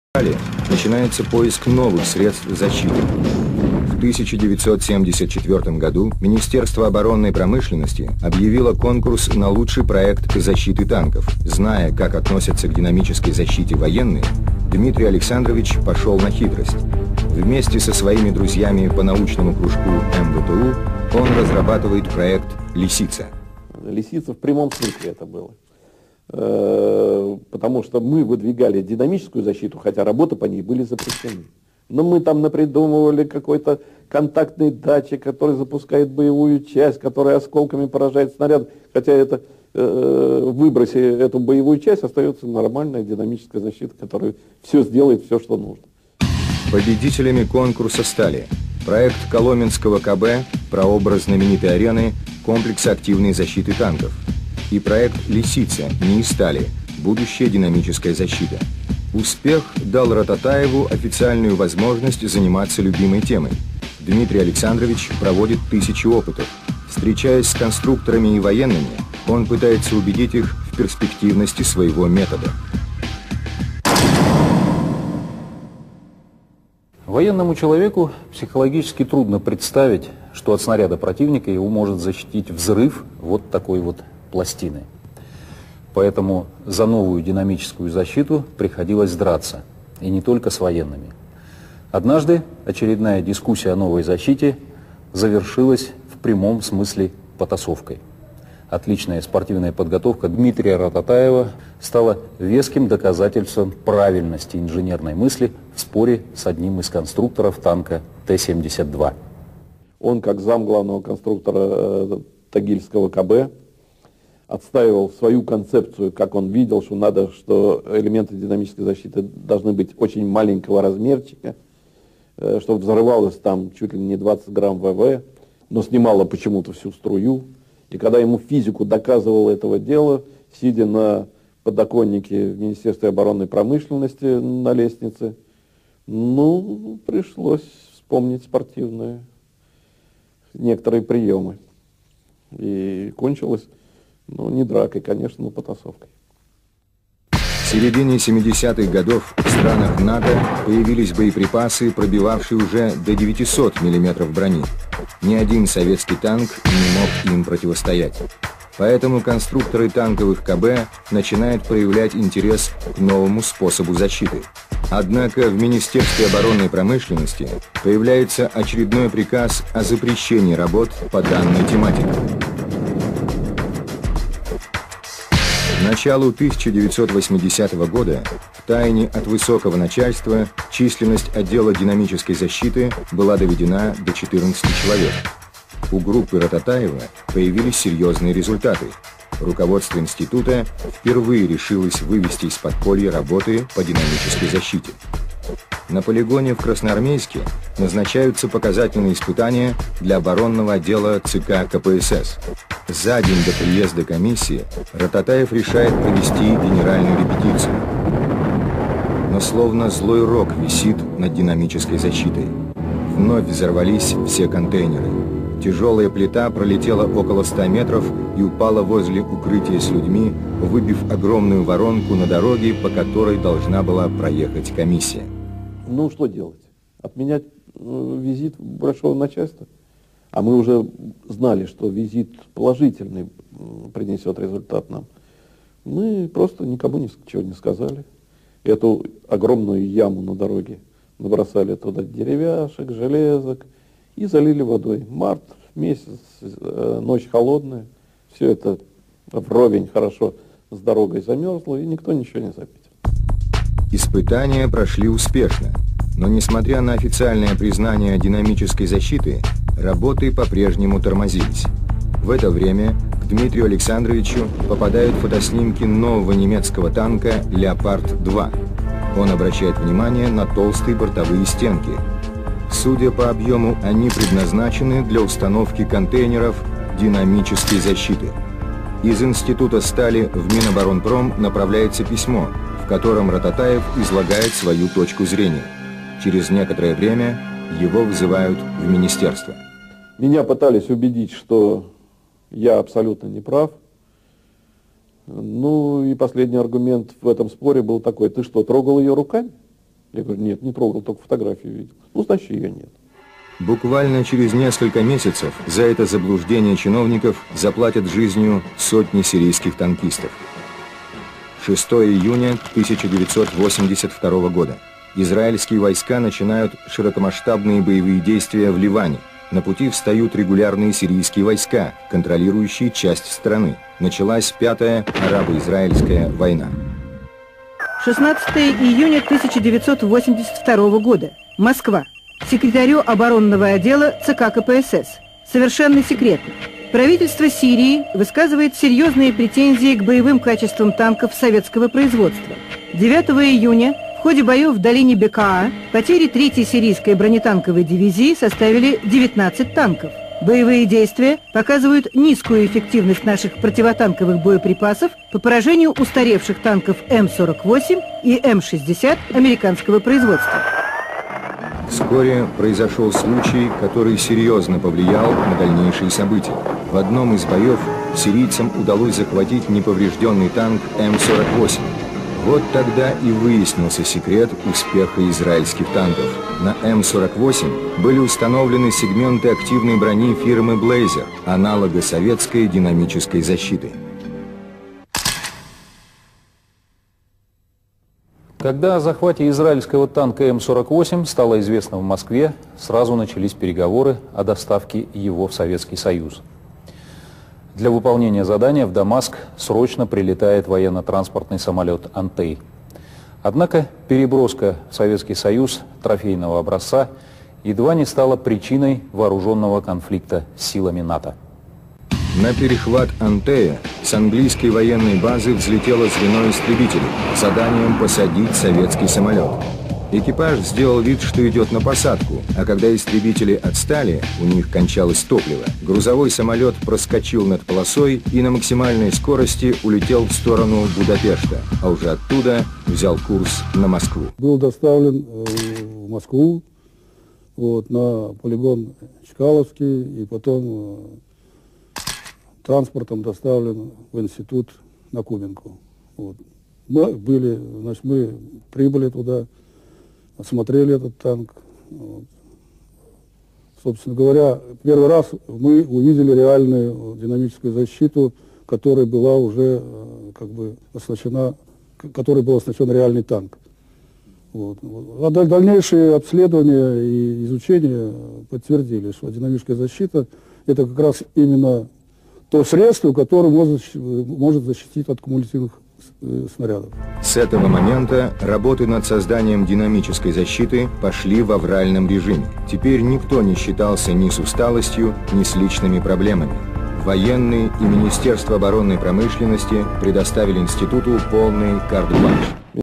Истоия ДЗ
К концу 70-х годов в Советском Союзе практически уже была отработана противокумулятивная броня, основанная на принципе контрвзрыва, работы над которой были начаты еще в 50-е годы. Ее внедрению